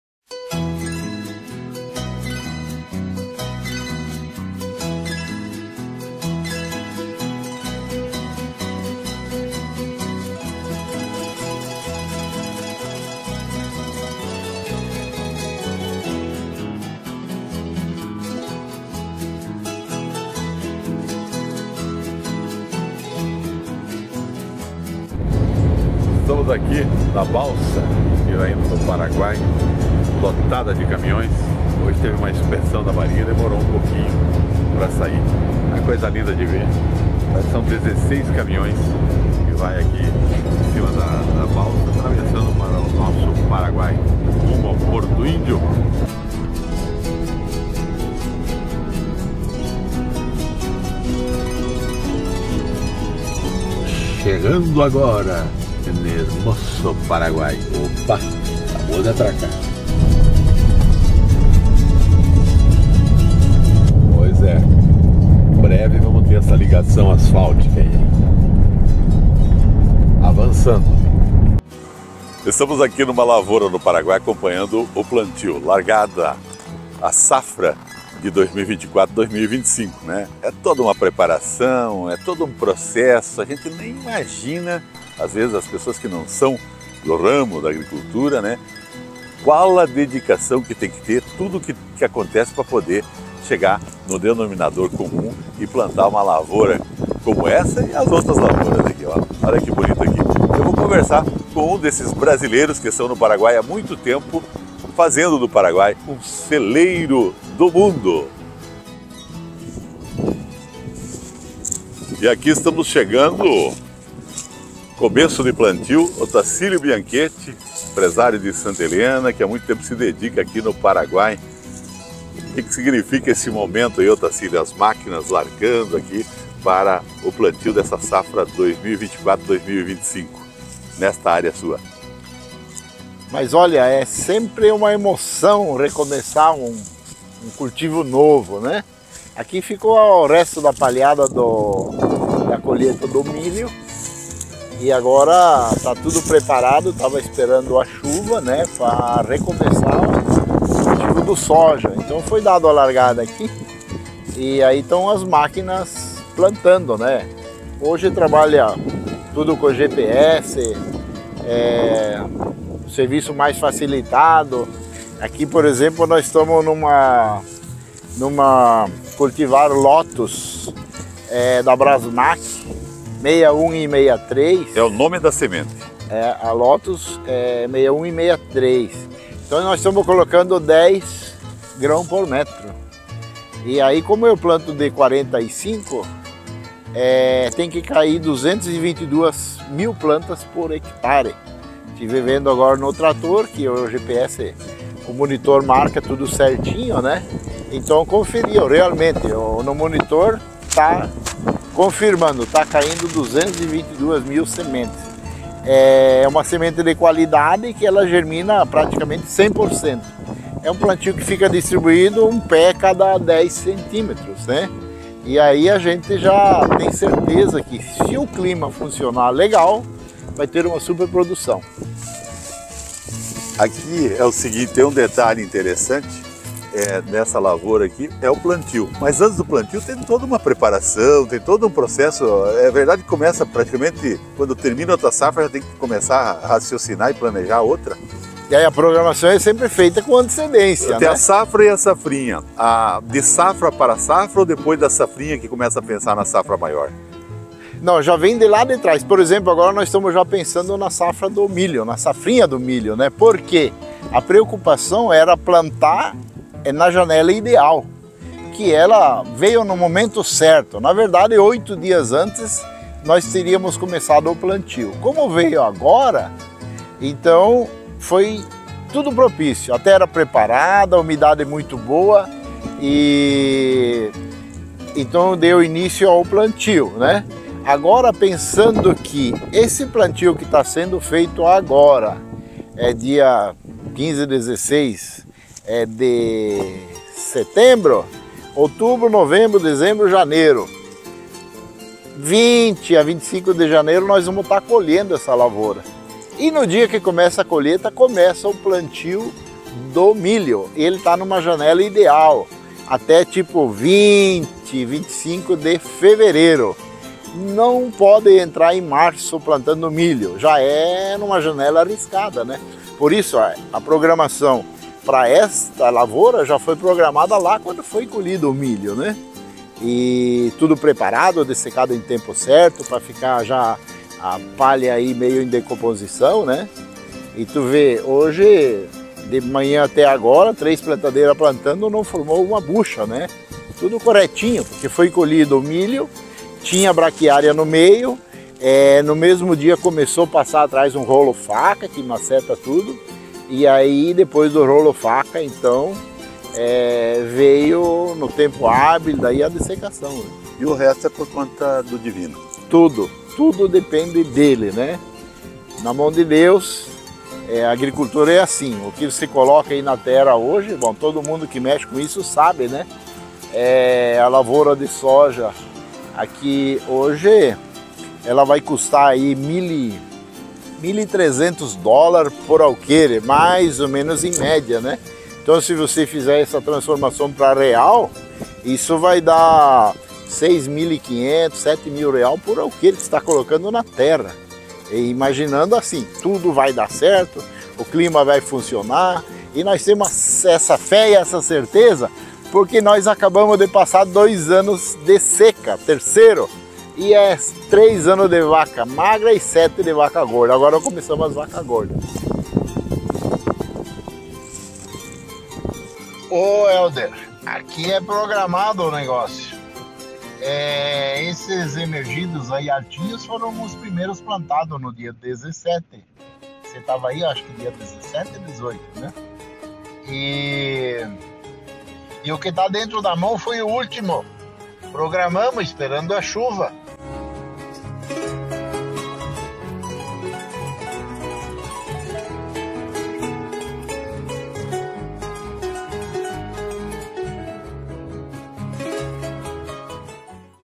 Brasiguaios e a plantação de soja no Paraguai. Safra 2024/2025.
Porto Internacional de Santa Helena.
Travessia Brasil/Paraguai.
Costa Oeste do Paraná.